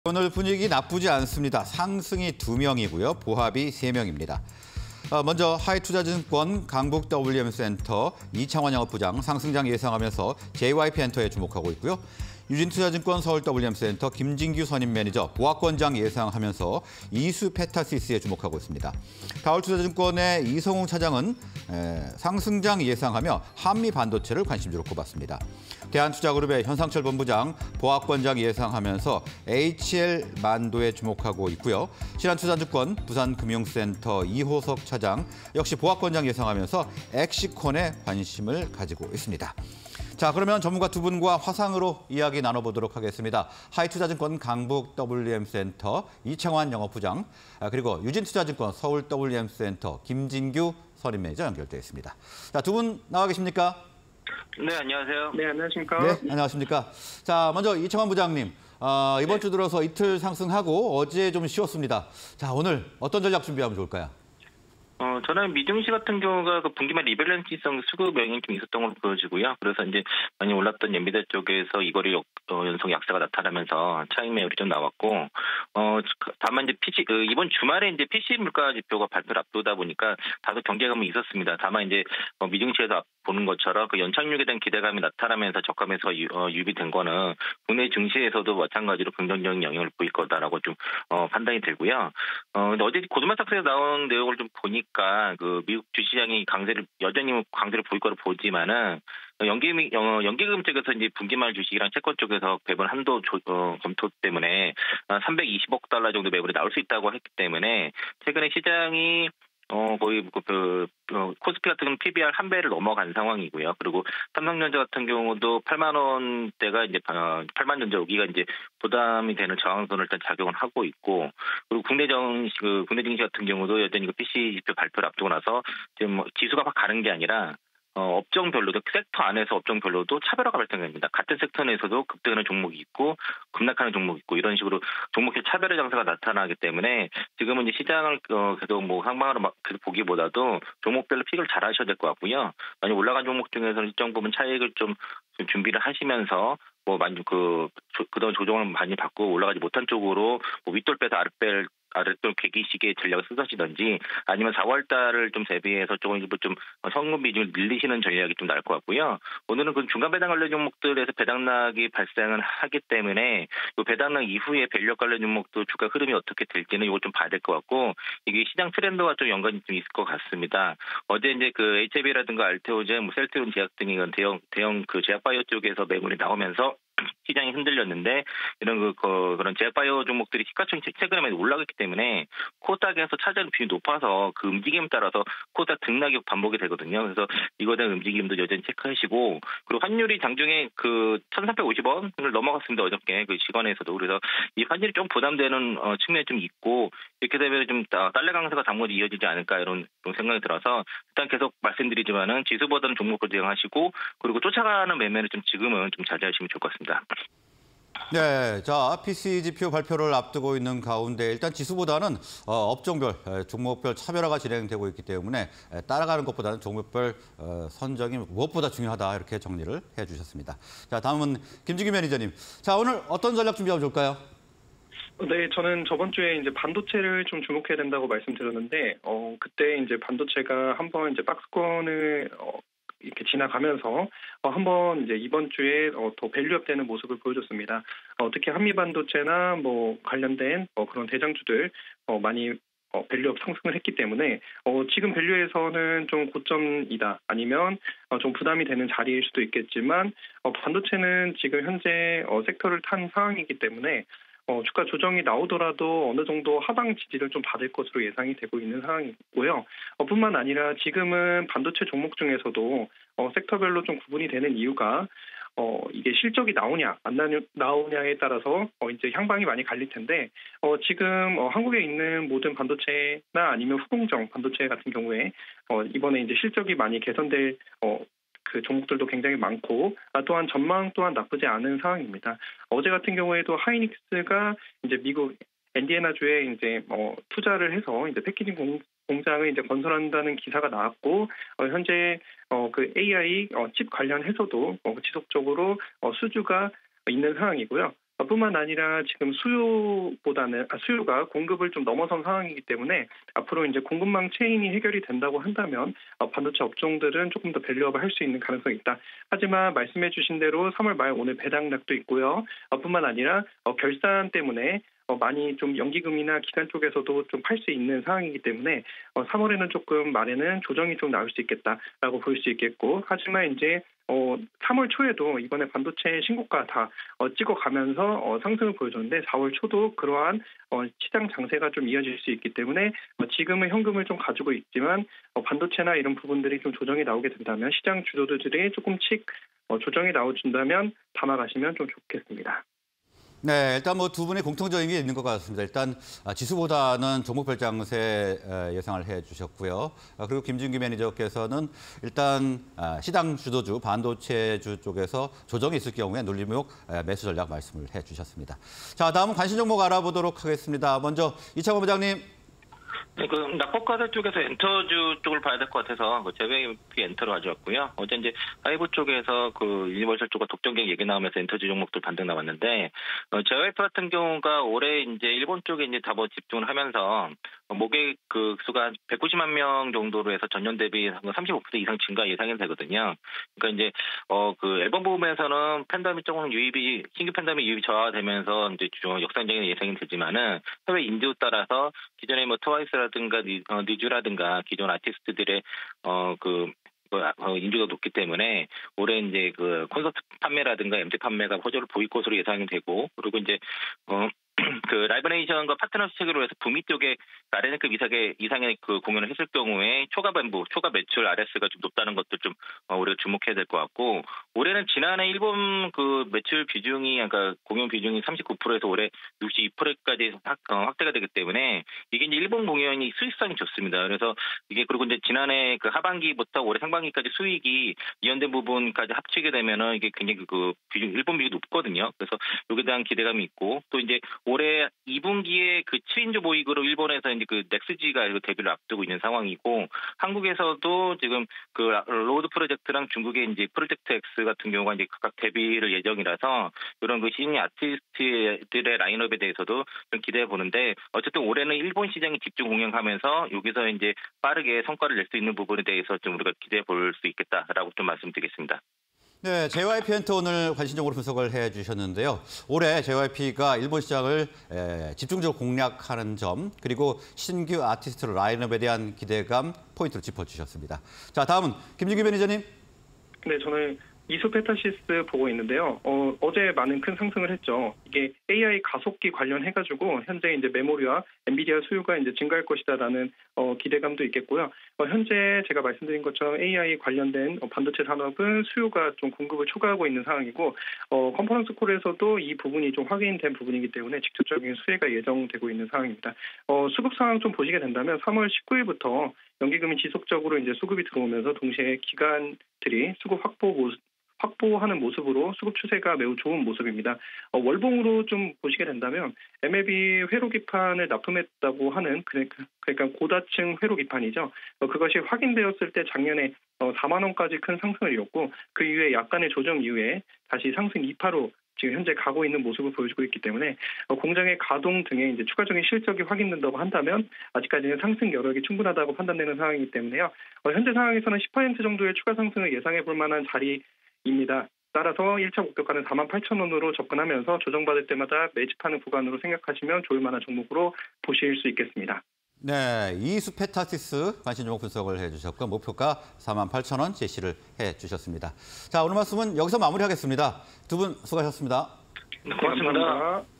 JYP Ent.(035900)... 연간 총 모객수 190만명…전년비 +35% / 이수페타시스... AI 가속기 관련 섹터 주도주_UP&DOWN 5인의 투자 전략 (20240328
Q. 숨고른 코스피, 개별 장세 지속되나?
Q. 2750선 지켜낸 코스피... 훈풍 이어질까?

JYP_Ent.(035900)
- 연간 총 모객수 190만명... 전년비 +35%
- 올해 일본 시장 집중 공략
- 신규 아티스트 라인업 기대

이수페타시스(007660)
- AI 가속기 관련 섹터 주도주
- 2024년·2025년 매출 성장 기대
- 수급 및 차트 흐름 양호